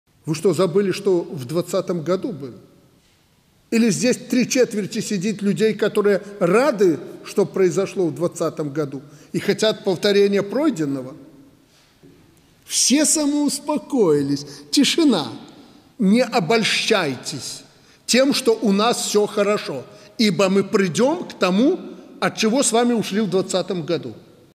Мы скоро придём к тому, что было в 2020 году! обещает лукашенко
19 апреля 2022 года на сборище перепуганных прикорытников лукашенко предсказал возвращение и скорую победу "Беларуского Августа 2020".
Скорее бы уже, а то уж заждались!
Жыве БЕЛАРУСЬ!